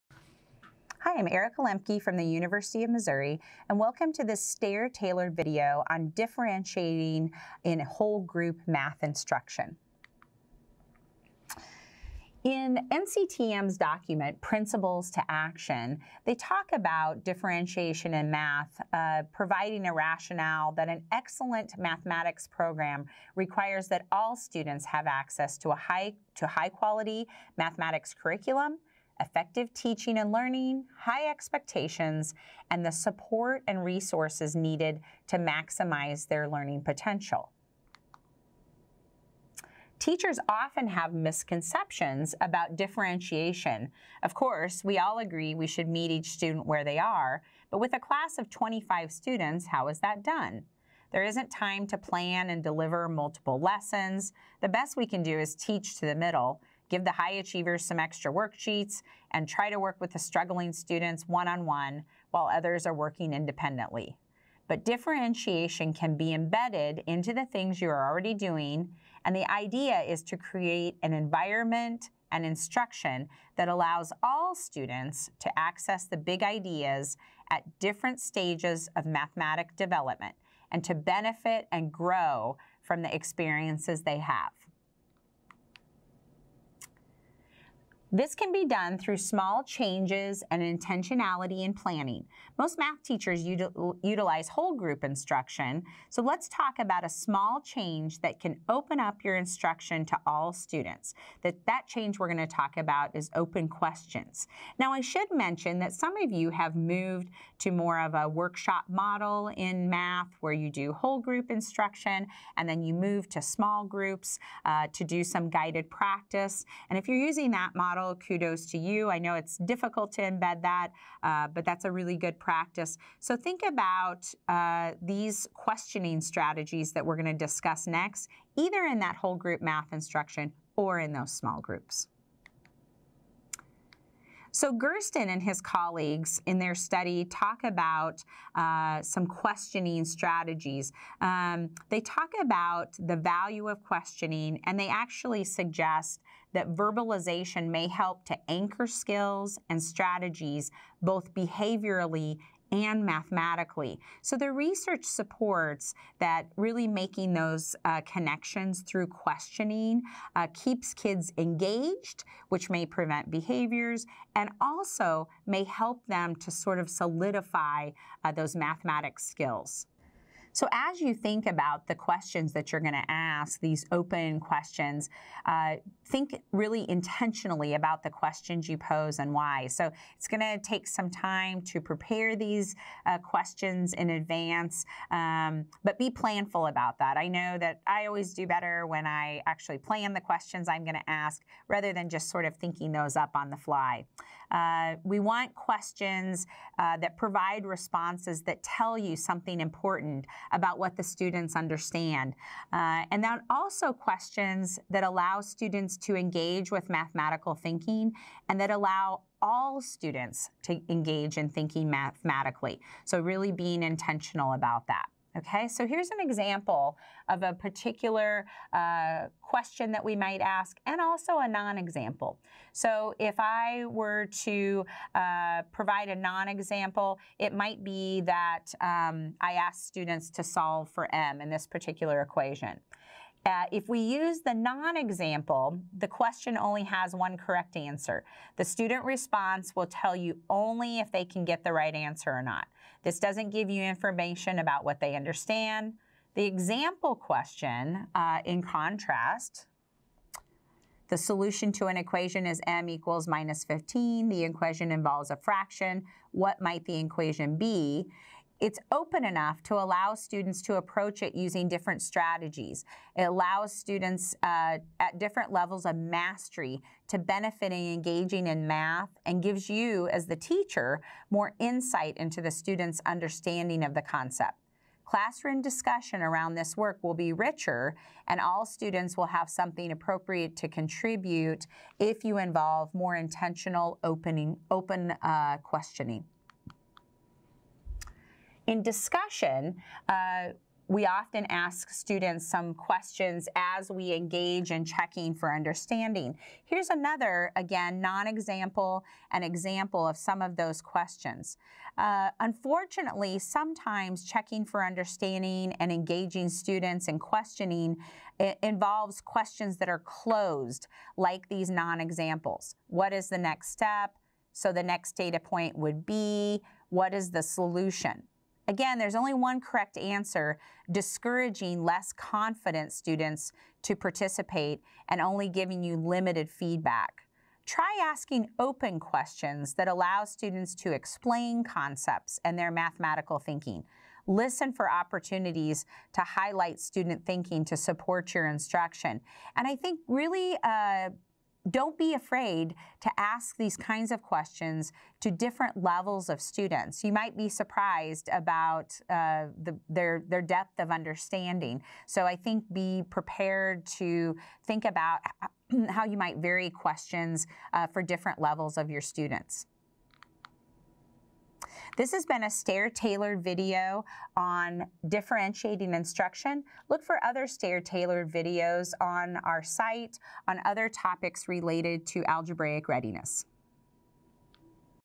Differentiating in Whole Group ‖ Best Practices ‖ Grades K-12 ‖ Project STAIR
Erica Lembke from the University of Missouri demonstrates ways that teachers can differentiate their math instruction for all their students by using planned and systematic questioning.